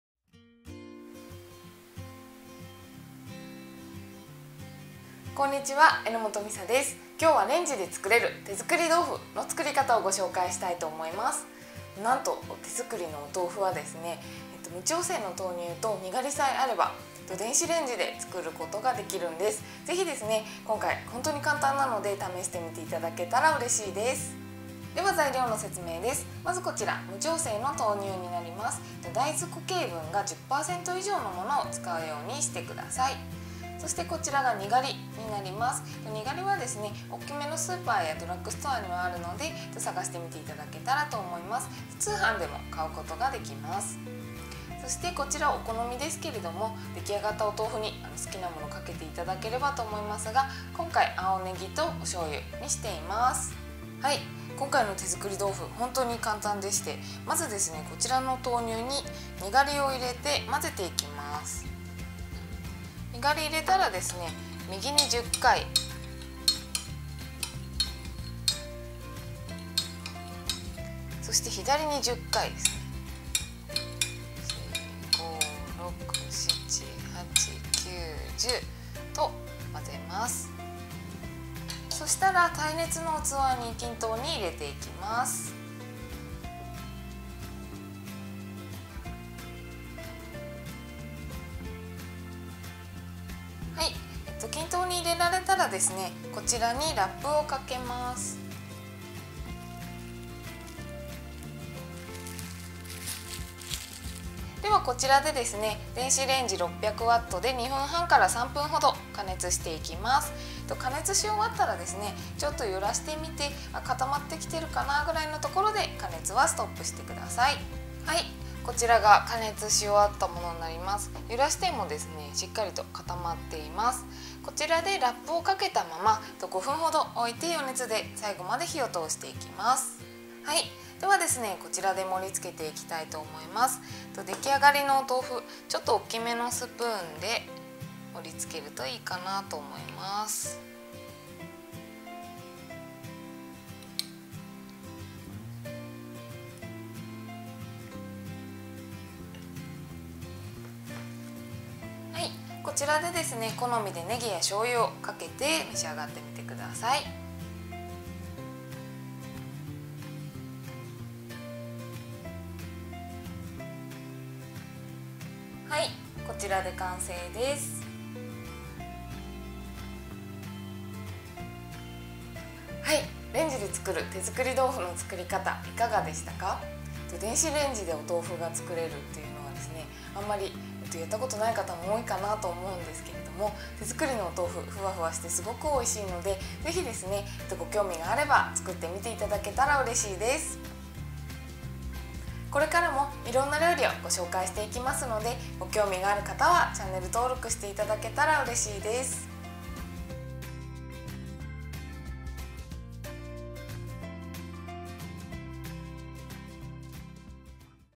レンジでつくる、手作り豆腐のレシピ・作り方
無調整豆乳とにがりがあれば、電子レンジで簡単にお豆腐がつくれます。
できたてのお豆腐は、ふわふわあったかでおいしいので、
ぜひあたたかいうちに召し上がってくださいね。

● Handmade tofu made with a microwave

■ Material
Soy milk (no adjustment / soybean solid content of 10% or more) 1 and 1/2 cup
1 teaspoon "nigari"
Green onion (small bite) appropriate amount
Soy sauce

[1] Put soy milk and "nigari" in a container and mix 10 times each.

[2] Put 1 in half in a heat-resistant container, wrap gently and heat in a microwave oven at 600W for about 2 minutes and a half.

[3] If it is solid, leave it on the wrap for about 5 minutes and put it in a bowl.